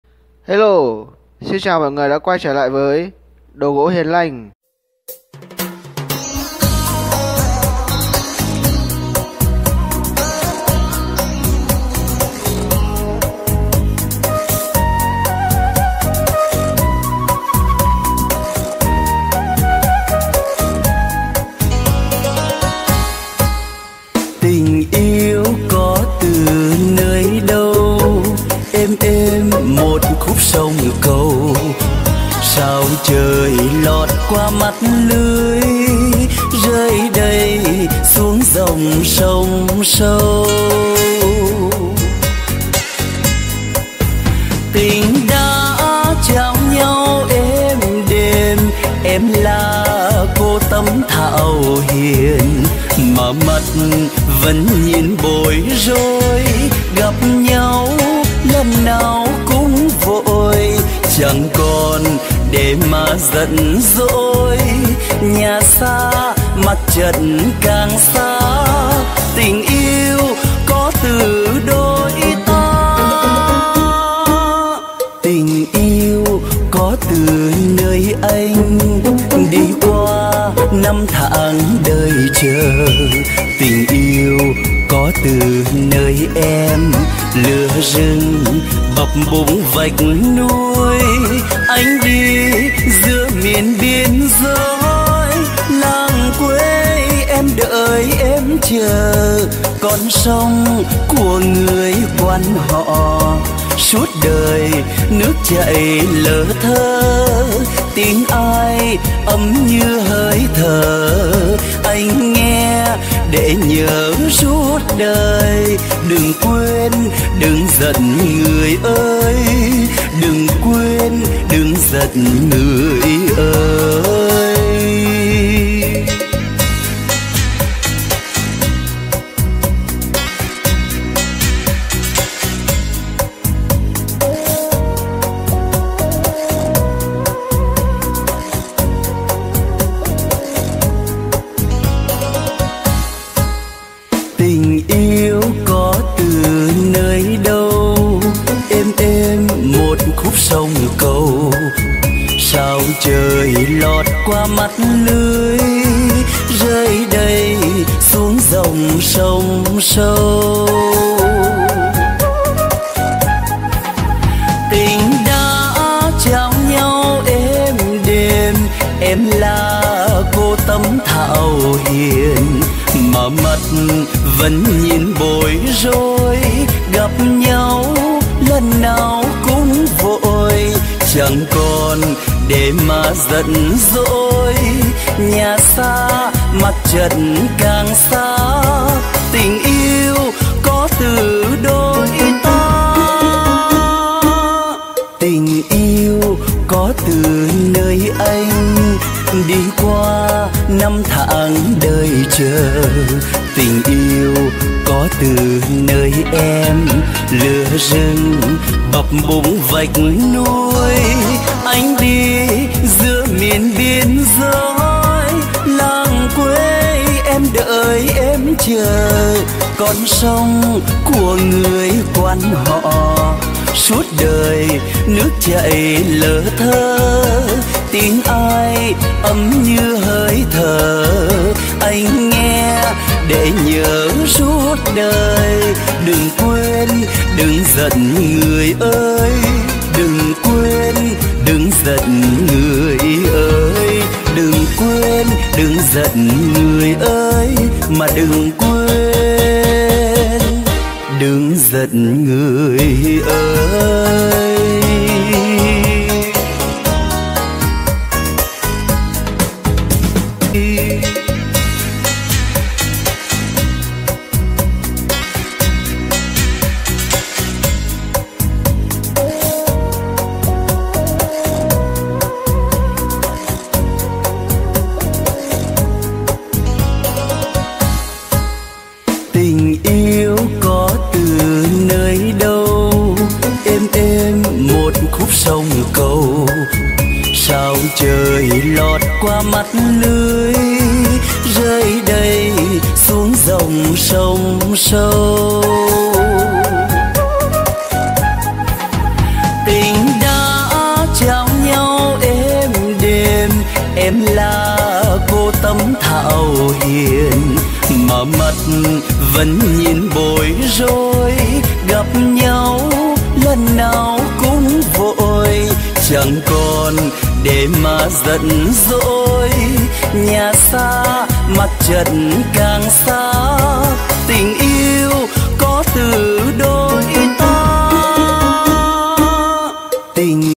Trường kỷ gỗ gụ chọn vân núi| trường kỷ đại gỗ gụ. đồ gỗ Hiền Lanh. 0986883349
Trường kỷ gỗ gụ chọn vân núi gồm 3 món(   2 ghế dài 2m17 & 1 bàn to dài 1m27 x 55)
100% được làm bằng chất liệu gỗ gụ
Trường kỷ gỗ gụ rẻ hơn trường kỷ gỗ hương đá